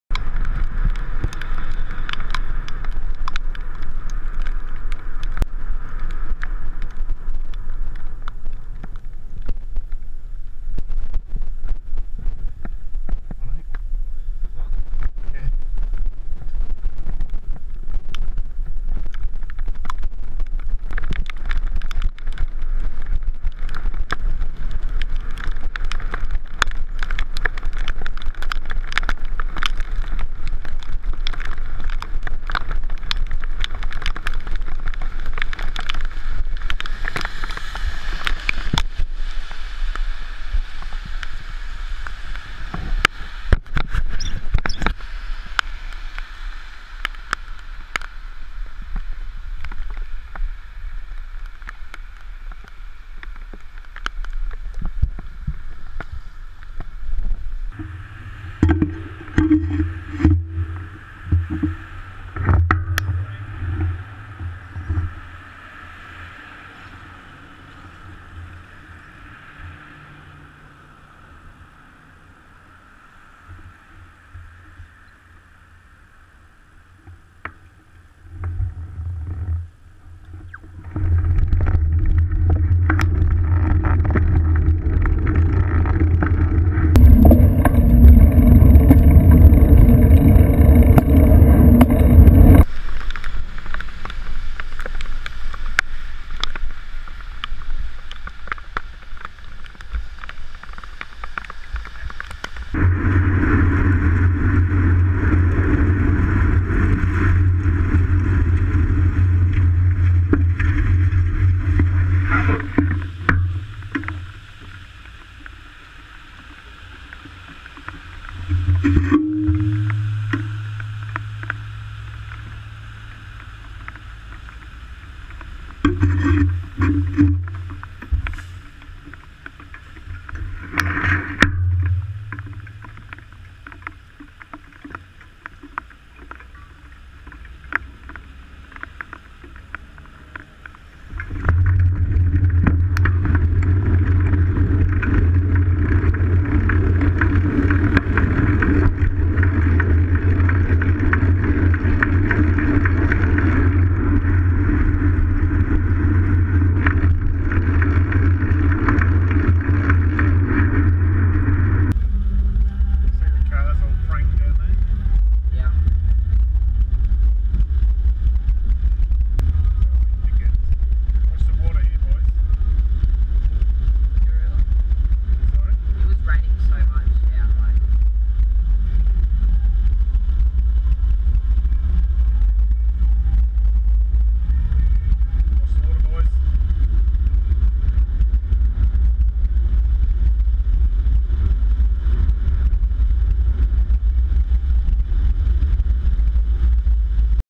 Am I Not Belgian Enough
It rained a lot today (a rare event).  There was some discussion on being Belgian (riding when it is cold and wet).